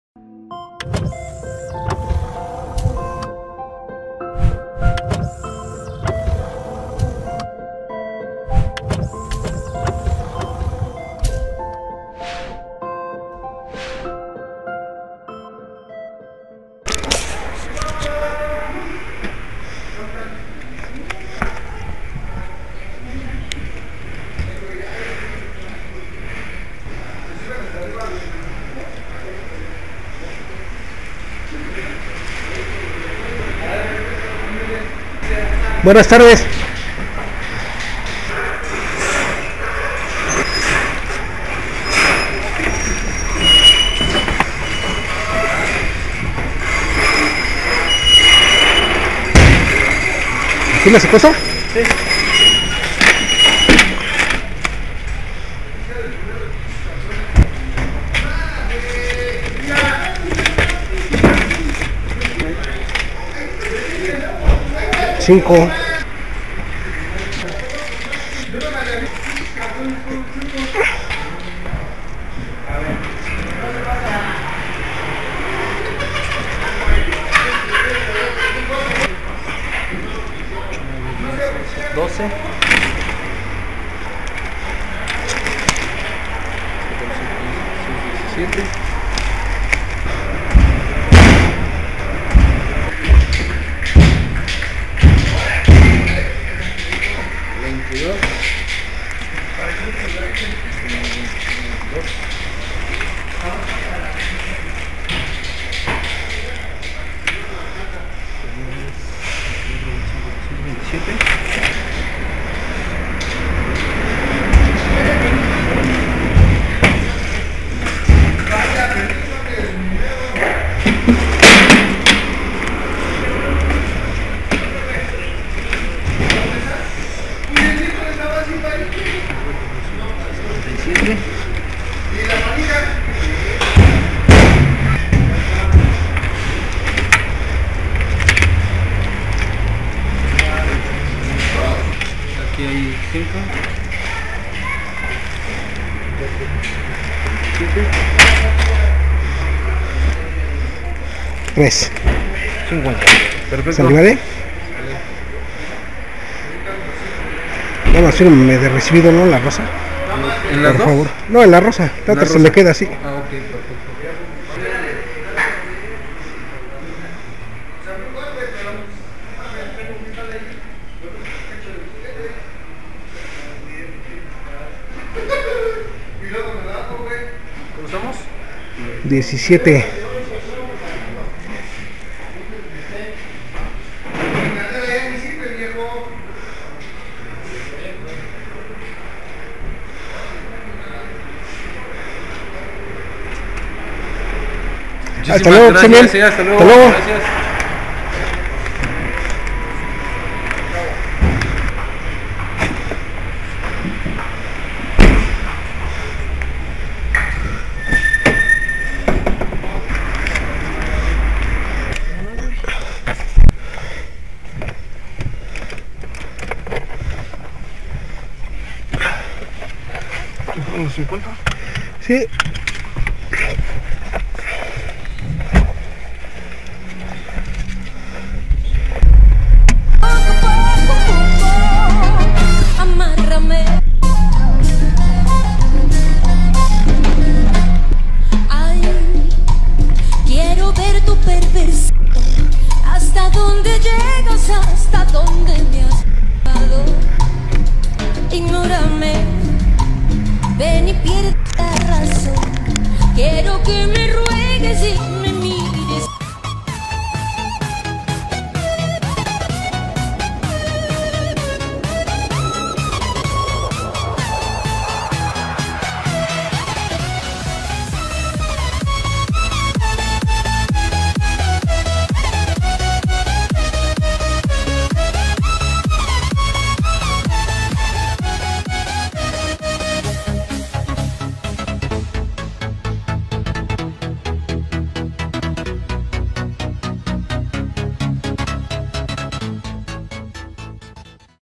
EN FRIEGA me traen
Correteado me traen  en tiempos de contingencia sanitaria, realizando  una entrega  mas  en otra  de las bodegas on line,  vamonos por el otro  que  esto  no acaba, recuerda NO SALGAS,  PAQUETERIA  TE LO LLEVA HASTA LA PUERTA DE TU CASA,  Saludotes desde la CDMX, dandole vida  a la  ciudad, El Lukas te saluda.